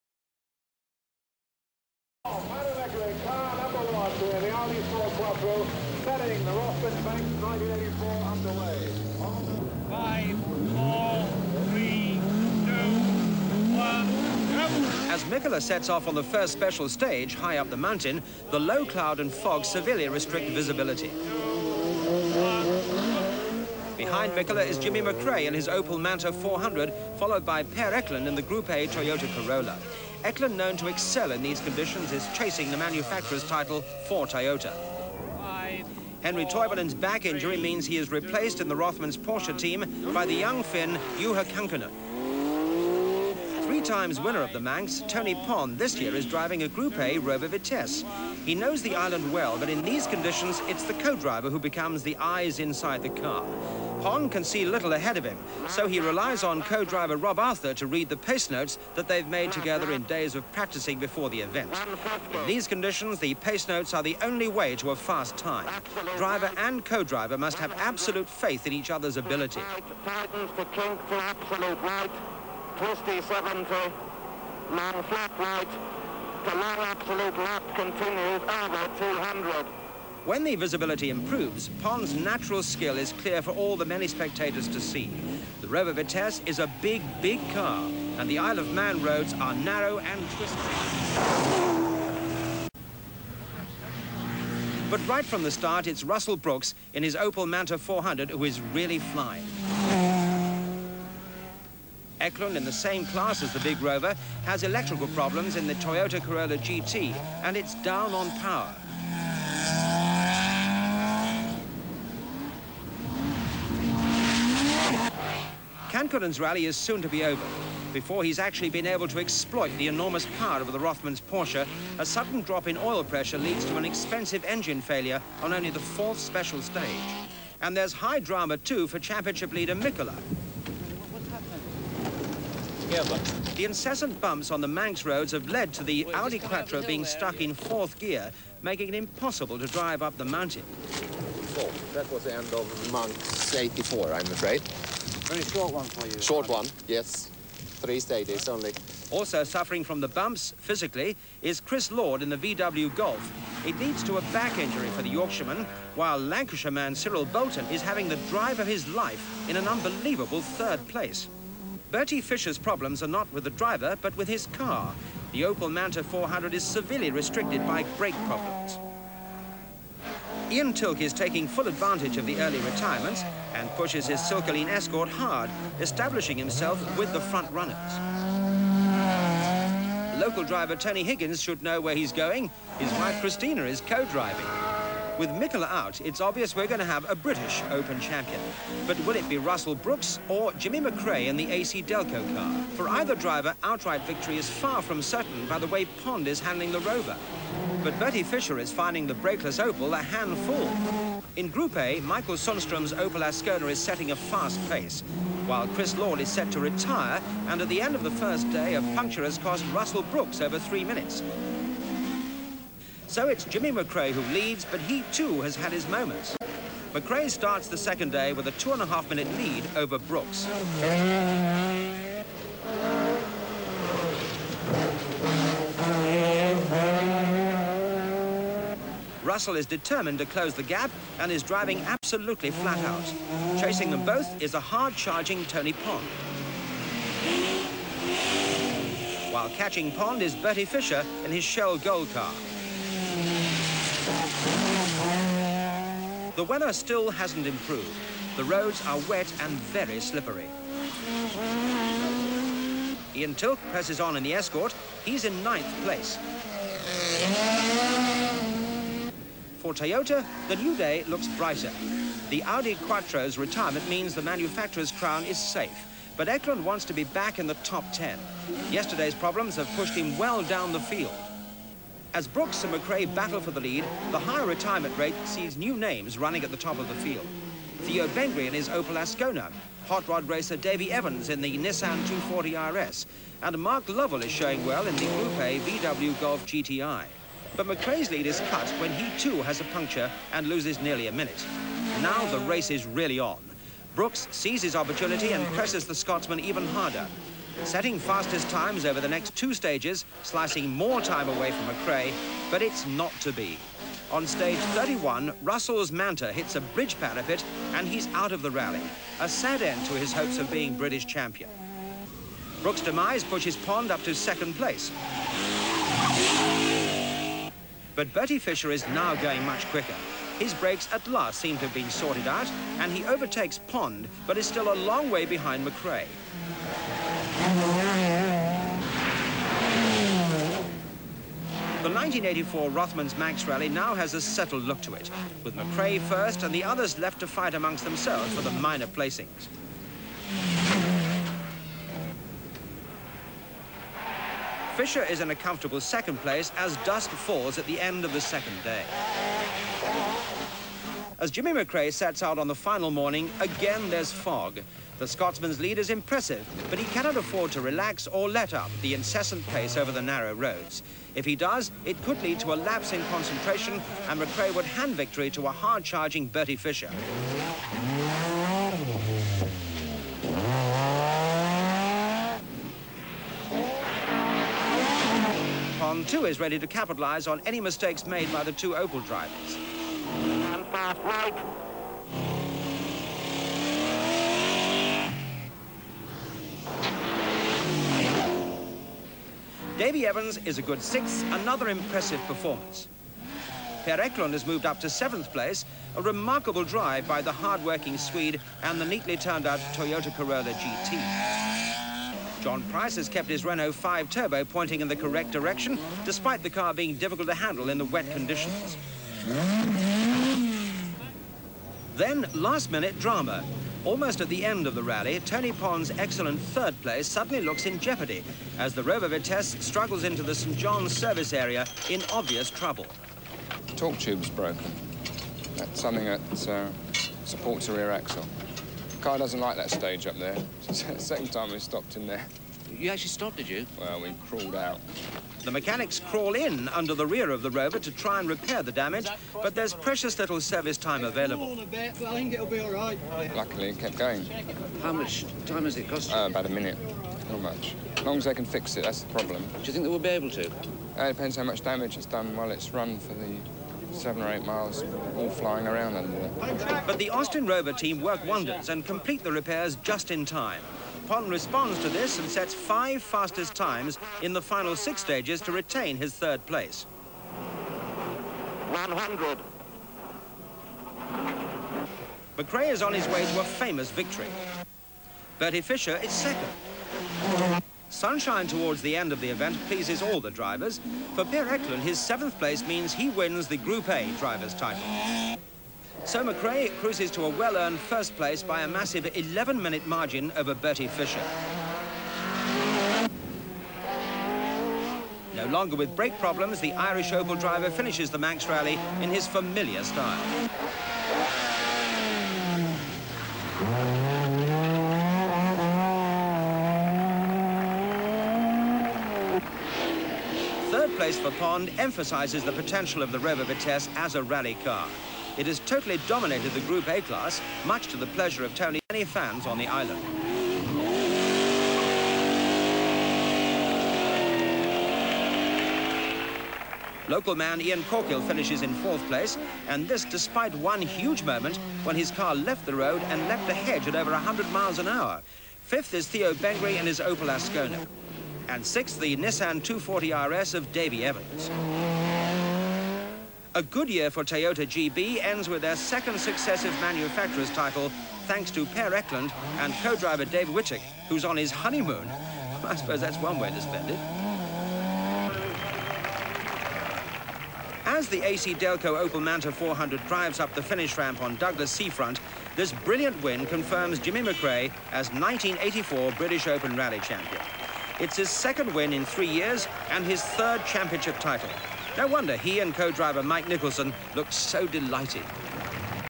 The 1984 Manx Rally
A video of the 1984 Manx rally. The rally and the championship was one by scotsman Jimmy mcrae in his AC Delco Opel Manta 400